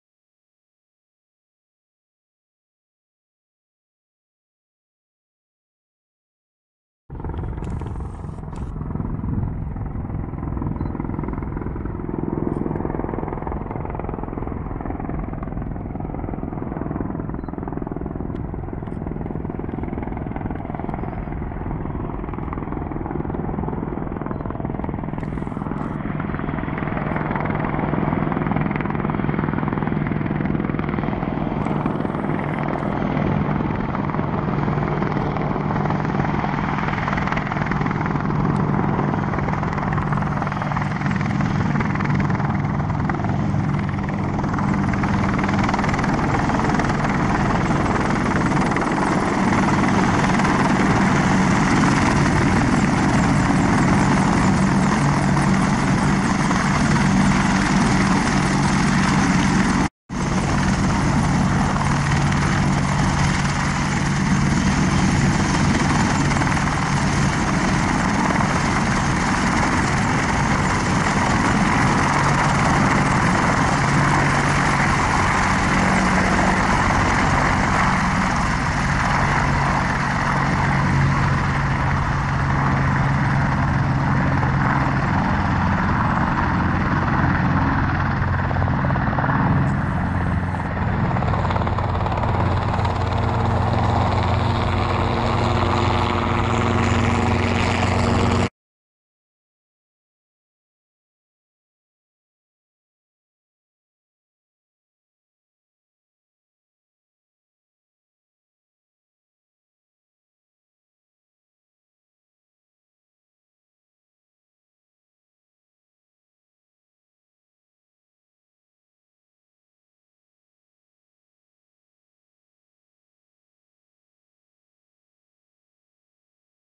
Apache visits Shobdon 8/4/22
ZJ211 visits Shobdon at 1552 on 8th April 2022. Nice.